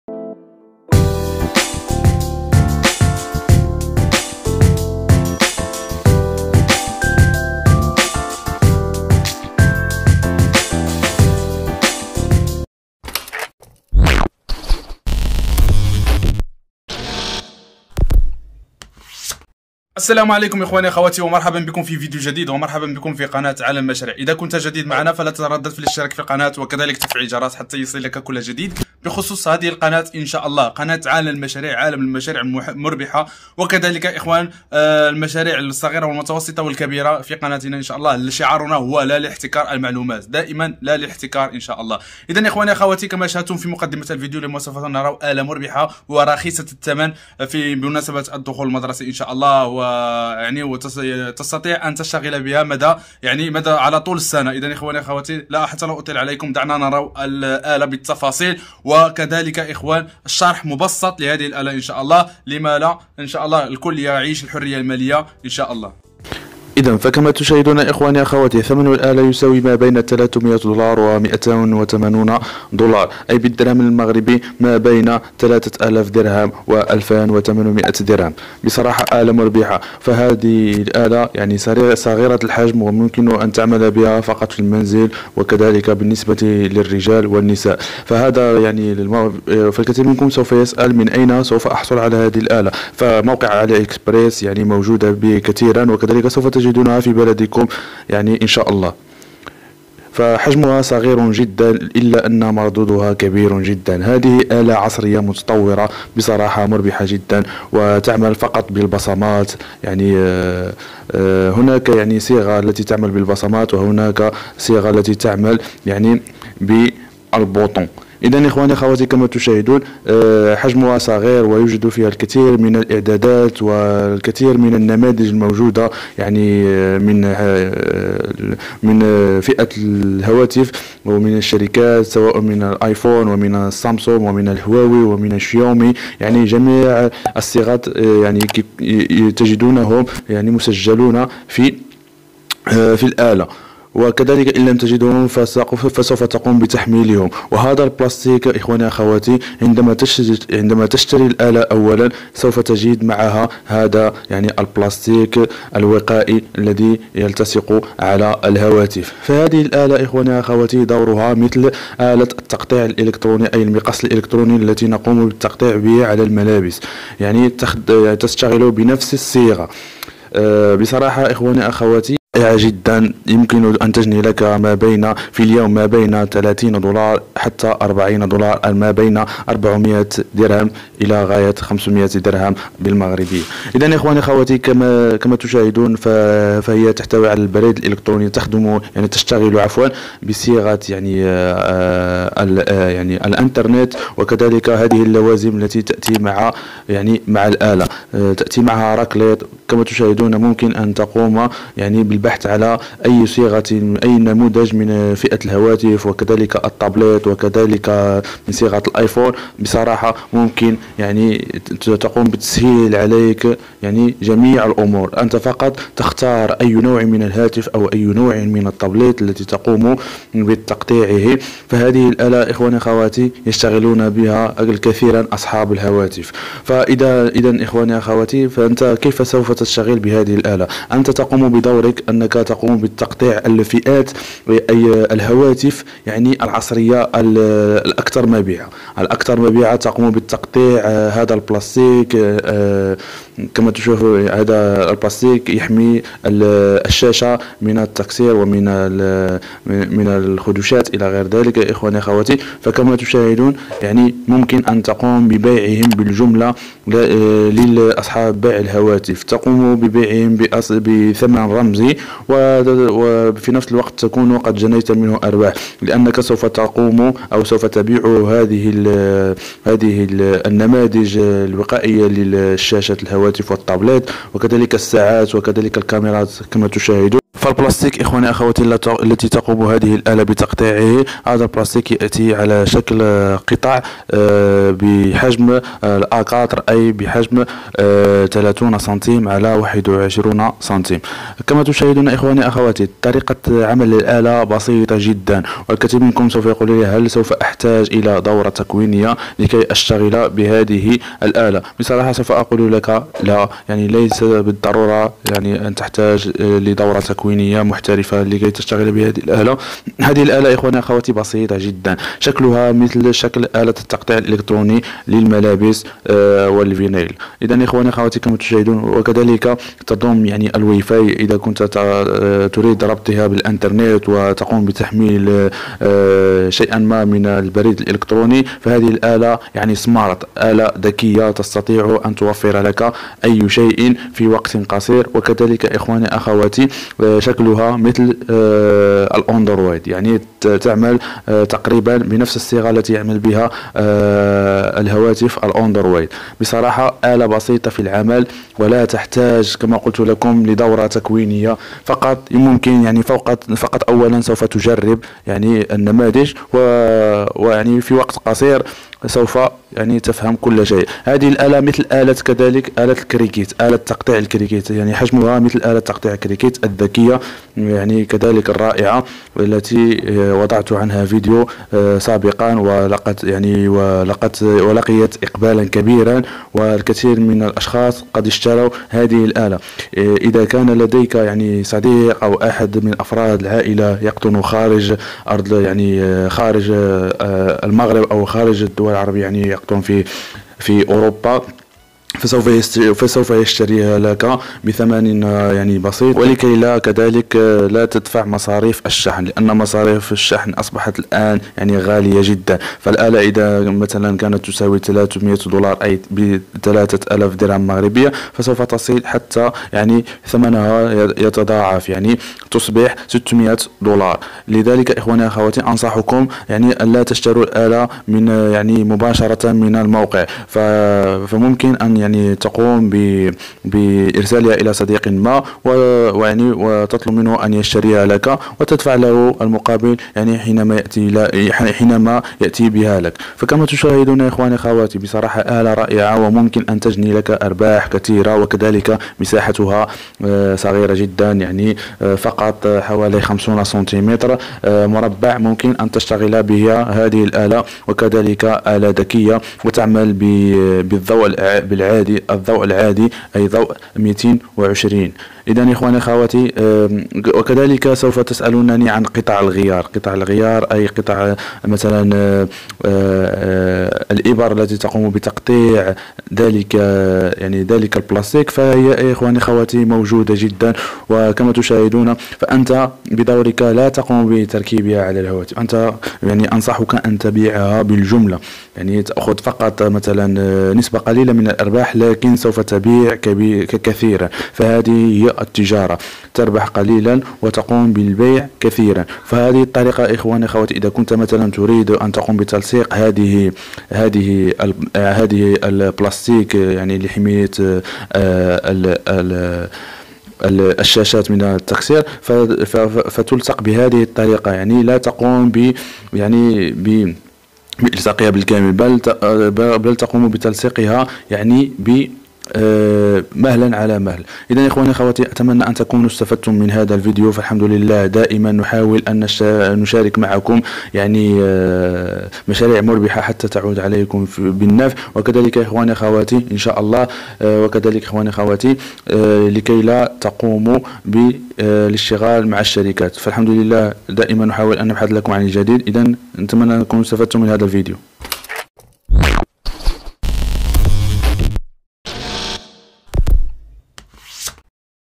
فكرة مشروع في المنزل و غير مكلف بدون تعب ربح أكثر من 50 دولار في يوم ... لن تندم شاهد ابدأ الآن
- الة الطباعة الحرارية
- الة طباعة الورق الحراري
- الة تقطيع الفليكس
- كومبيوتر
- ورق حراري
- برنامج الفوتوشوب
- برنامج اليسترايتور
- برنامج الكوريل دراو..